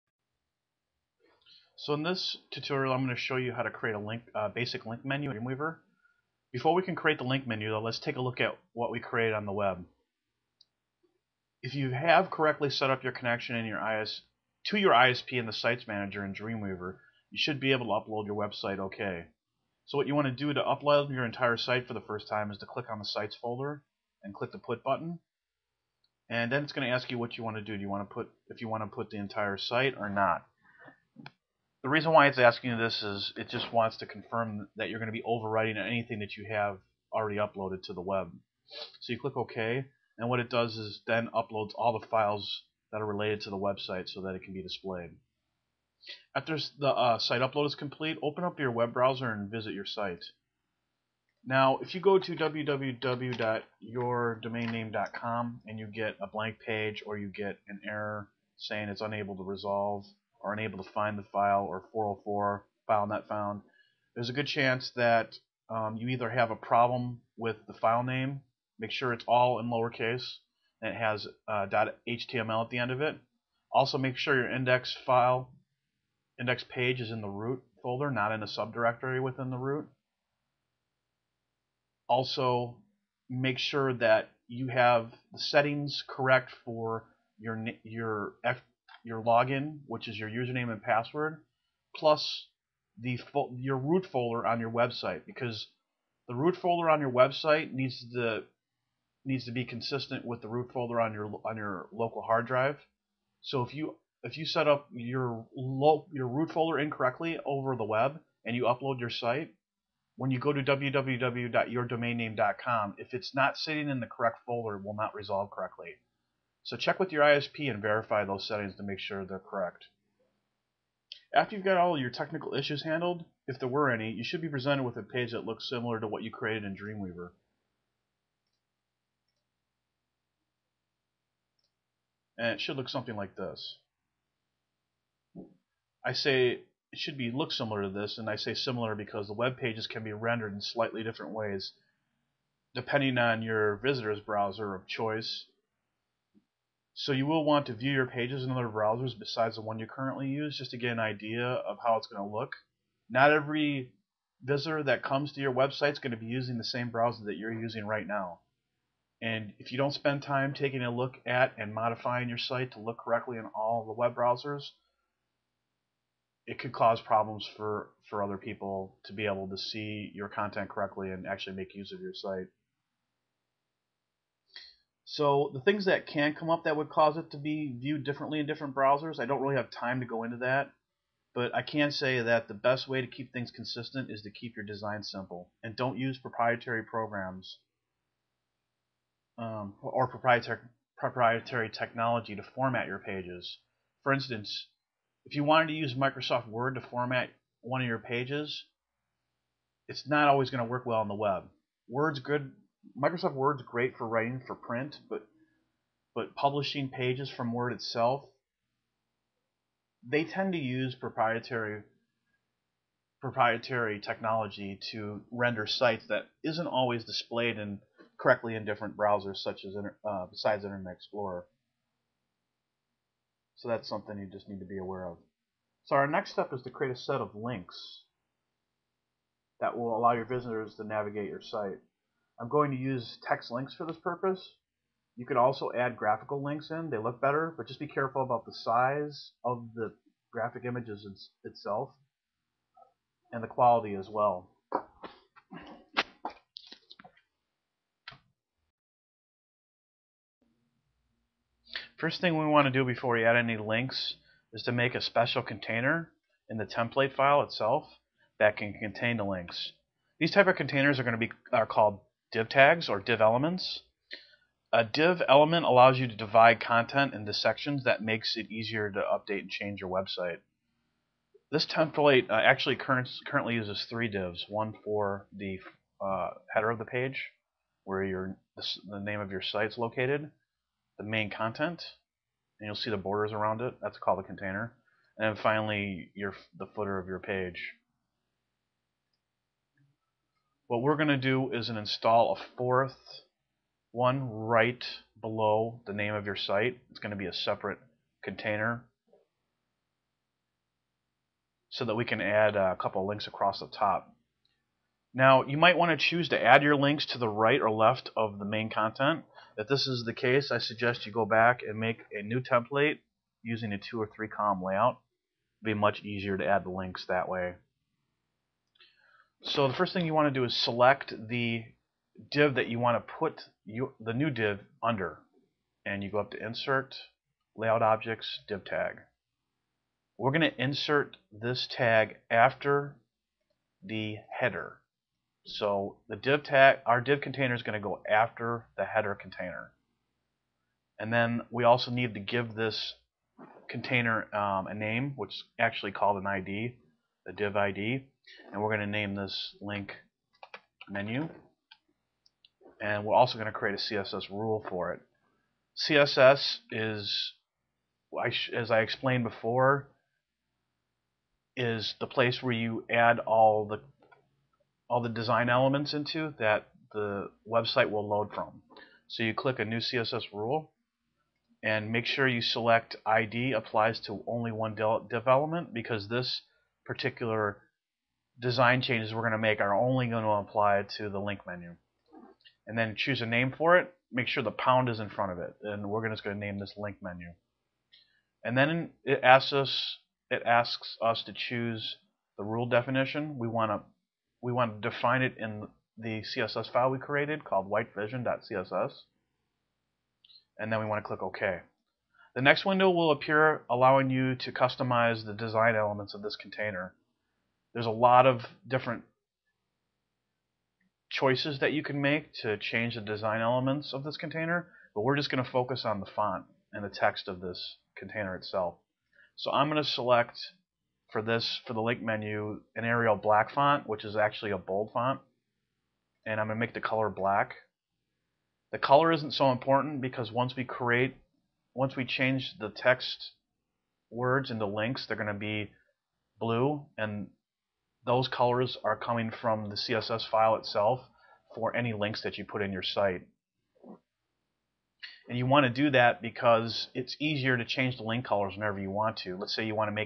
Dreamweaver CS3 Tutorials: Creating Menu Links Part 1
Scott Larson Consulting shows you how to add a new link menu using the built in template system. to learn more about my services.